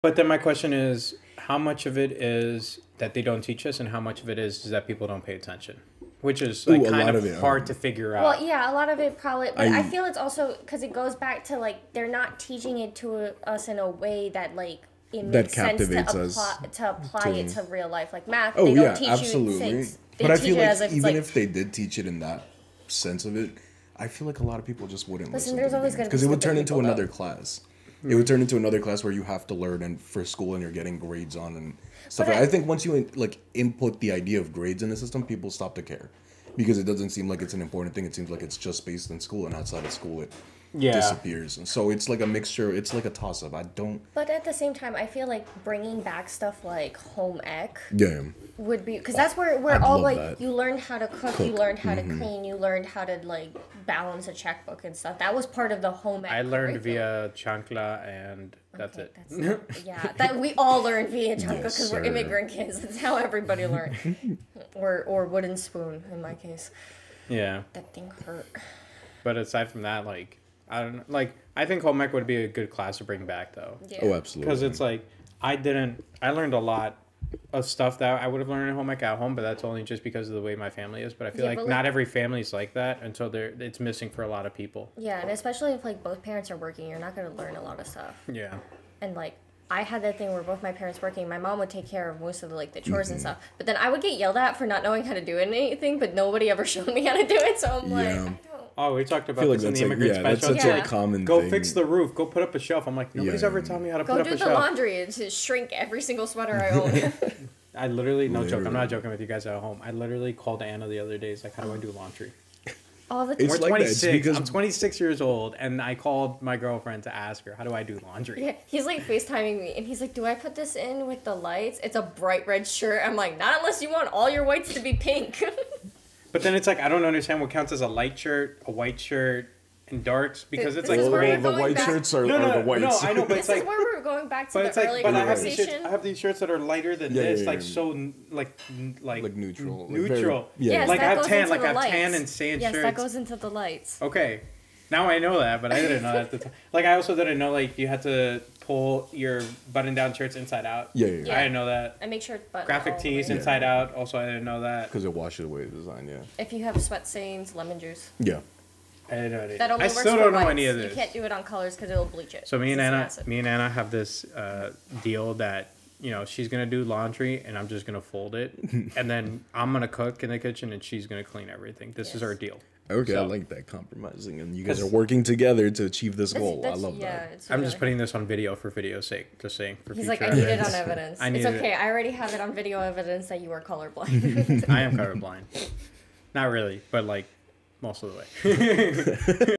BRING BACK HOME EC!!!
Brandon and Steven with special guest Anna, talk about bringing back home ec and the things they should have learned in high school.

Check out the full episode on the channel!

Episode 7 Things I Wished I Learned in High School